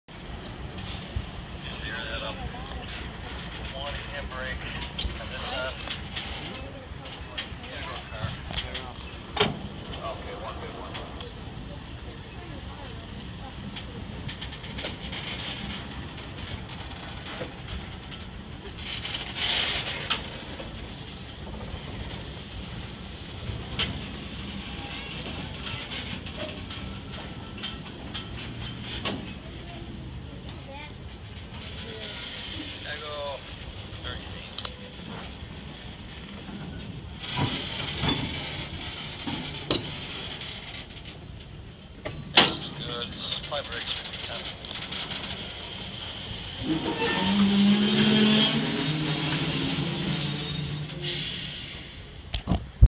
Fair train pulls forward and stopes
This is 5010 pulling forward